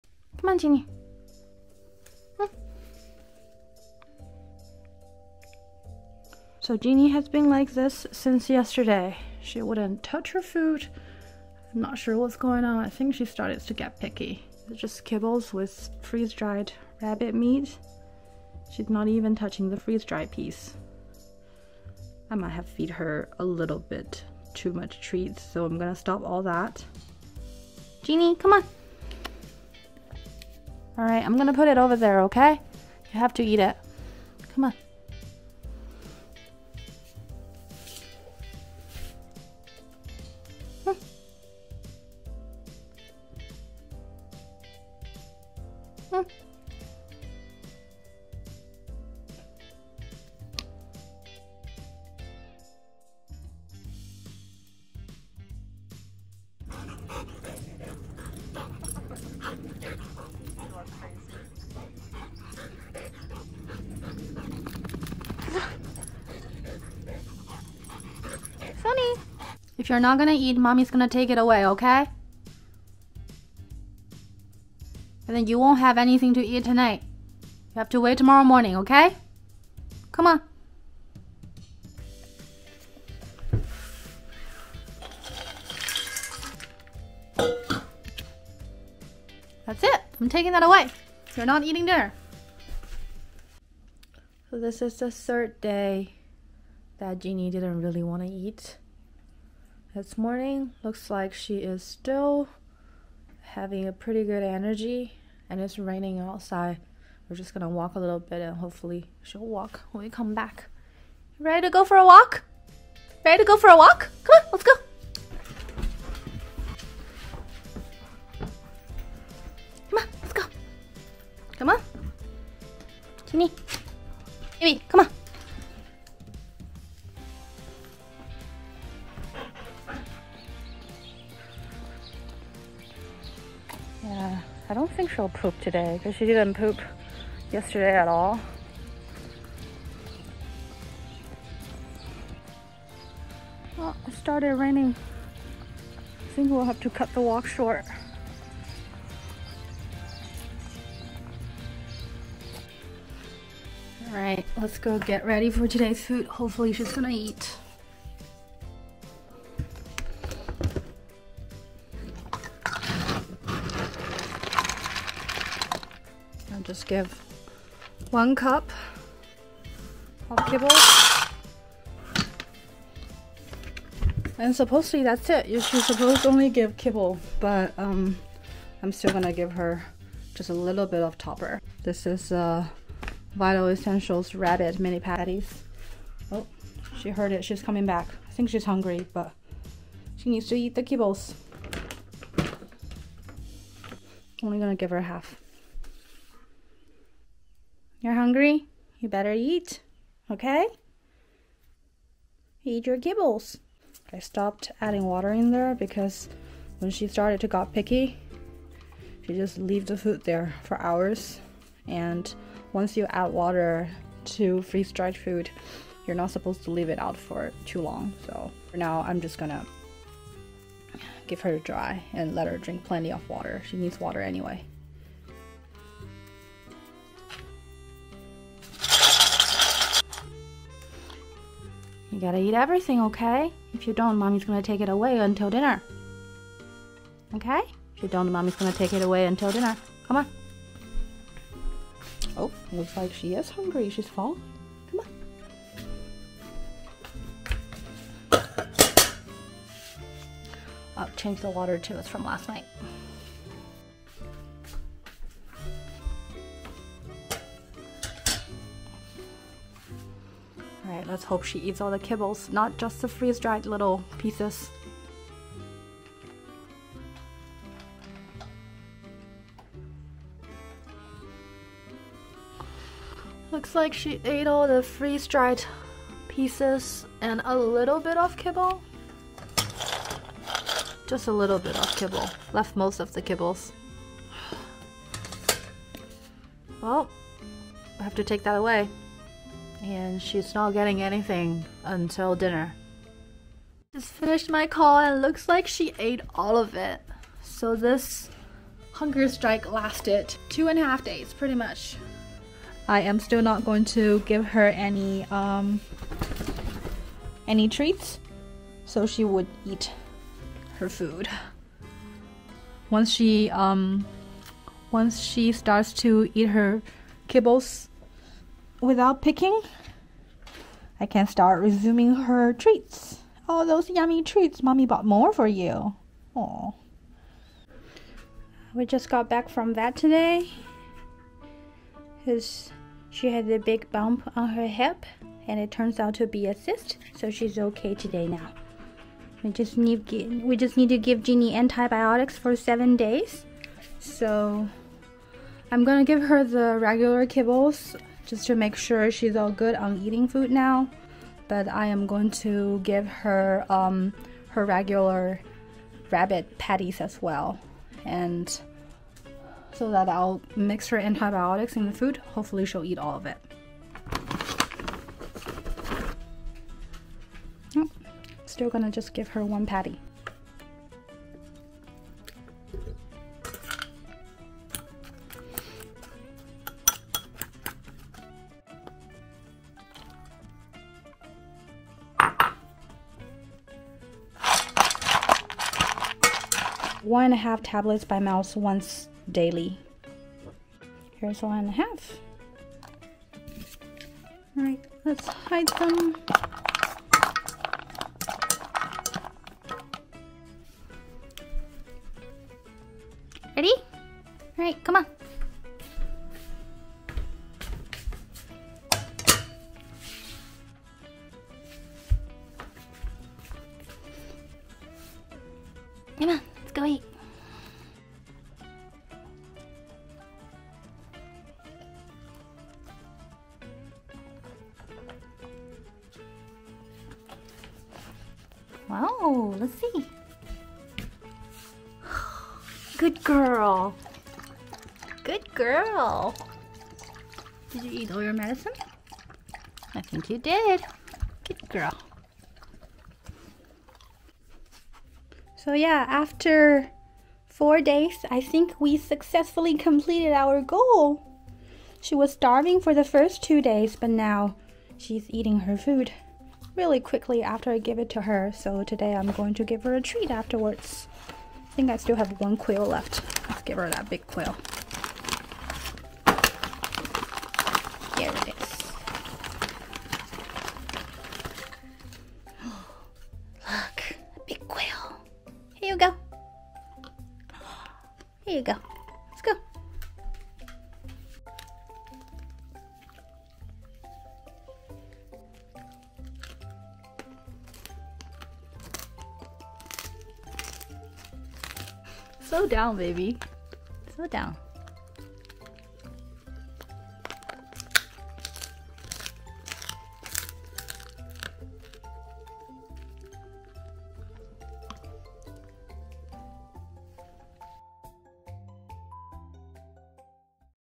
How To Get A Picky Dog To Eat Kibbles? I Did It In 4 Days!
Genie the Goldendoodle went on a mini hunger strike last week, probably because I gave her too many treats recently and now she refuses to eat her kibbles. I had to put her on a schedule to fix this unwanted behavior and to get her back on eating dry food again.

Genie's regular diet consists of kibbles + freeze dried meat, can food once in a while with occasional treats. She is a slow eater, I usually leave the food out for her so she can eat whenever. But on this scheduled diet, things has to be done a little differently, I will only give her 10 mins to eat, if she is not finished by the 10 min mark, I will take the food away from her. I only gave her about half of what she normally eats per meal, so she would be hungry for her next meal, in the meantime, make sure she gets enough exercise. It's ok for dogs to not eat for a few days, but make sure they drink plenty of water. If your dog does not eat or drink anything for a few days, please contact your vet.

Timestamp:
00:00 - 00:51 Intro and Day 2 breakfast
00:51 - 00:56 The scheduled diet
00:56 - 01:33 Day 2 dinner
01:33 - 04:53 Day 3 breakfast
04:53 - 05:27 Day 3 dinner
05:27 - 07:24 Day 4 breakfast
07:24 - 08:27 Treat time
08:27 - 08:30 Outro